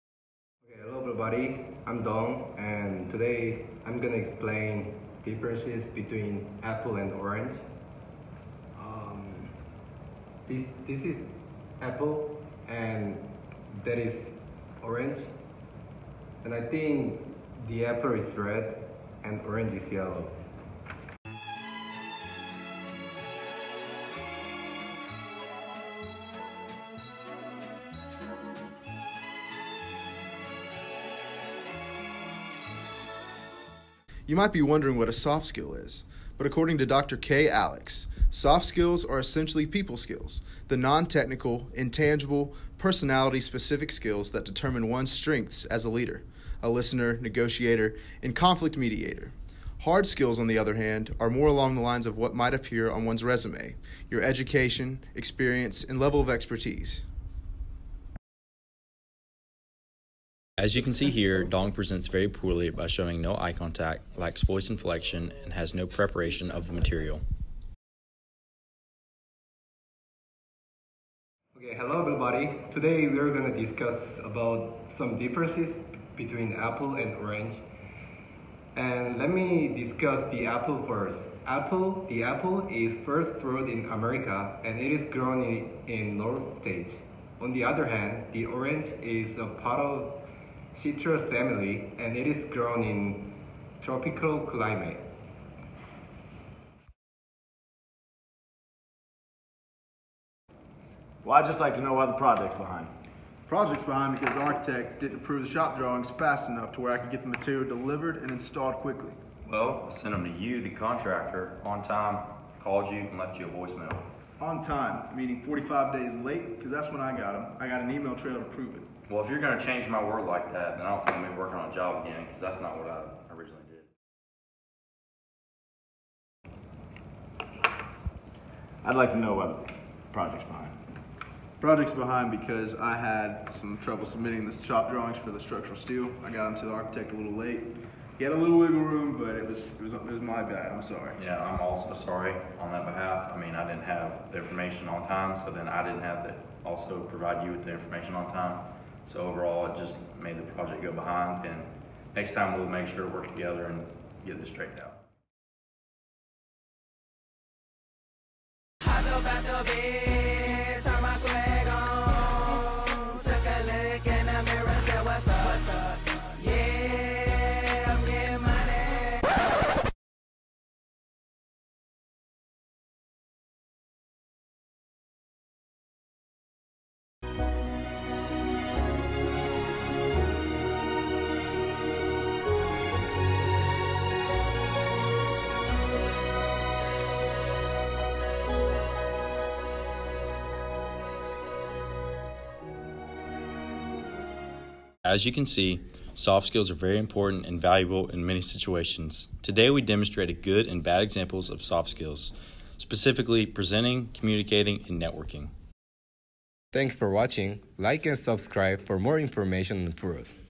Soft Skills in a Formal Presentation BSCI 4610
Dong Han, Luke Furtick, Josh Reid, Chase Brewer
Dr. Wetzel BSCI 4610 Scheduling